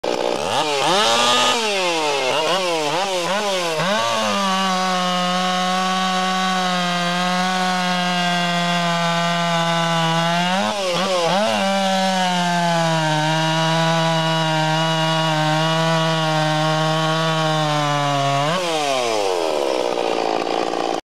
MMWS PORTED MS440/MS460 HYBRID
Clint's Hybrid tuned to 14,800 RPM